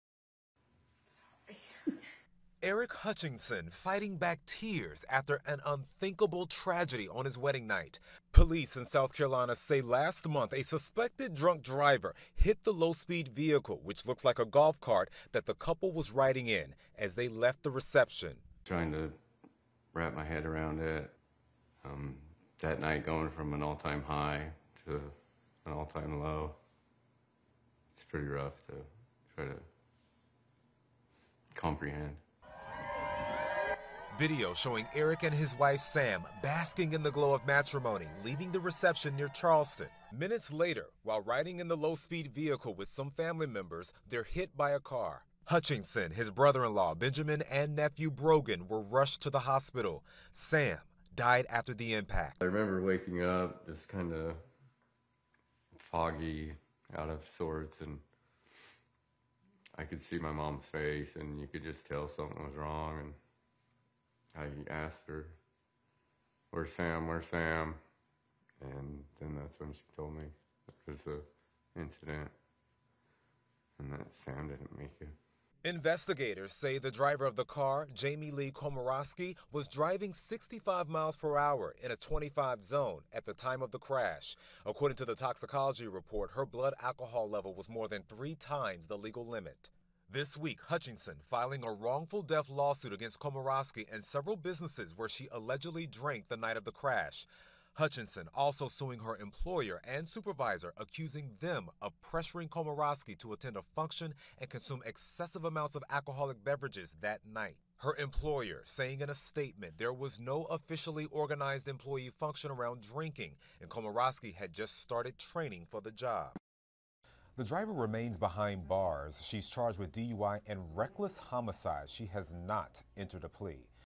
Groom of bride killed on wedding night shares his story: 'She wanted the night to never end'
A South Carolina groom whose bride was killed on their wedding day by an alleged drunk driver is sharing his story for the first time.